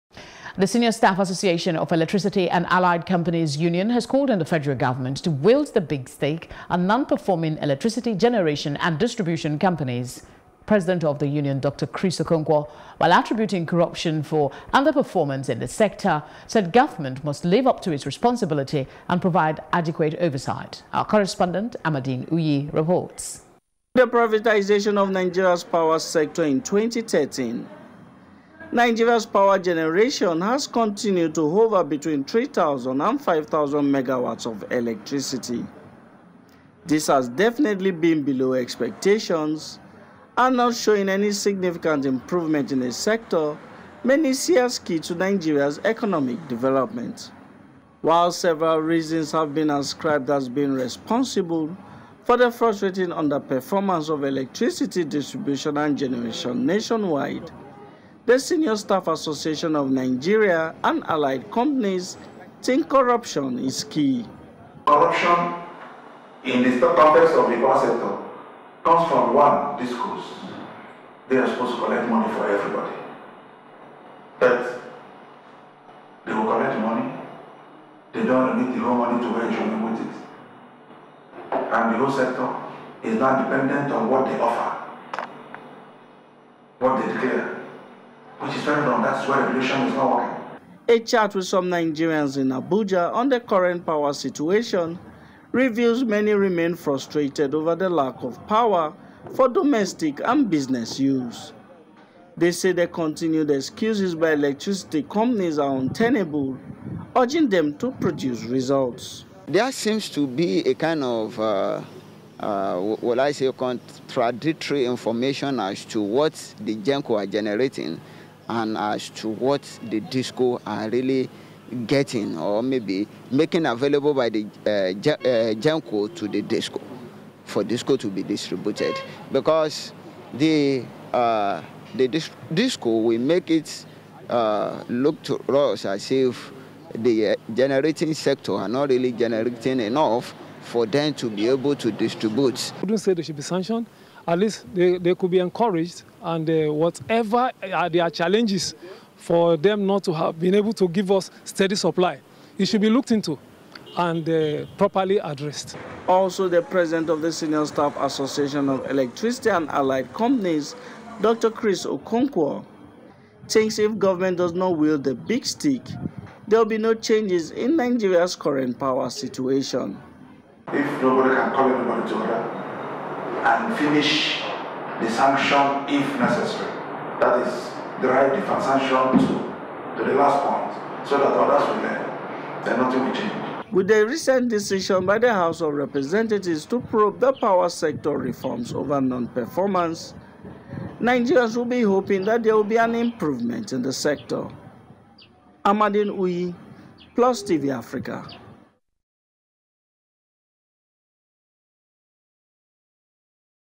Power Sector Reforms: Union Asks FG To Sanction GENCOS & DISCOS Over Non Performance
The Senior Staff Association of Electricity and Allied Companies Union has called on the federal government to wield the big stick on non-performing electricity generation and distribution companies.

Our correspondent Amadin Uyi reports.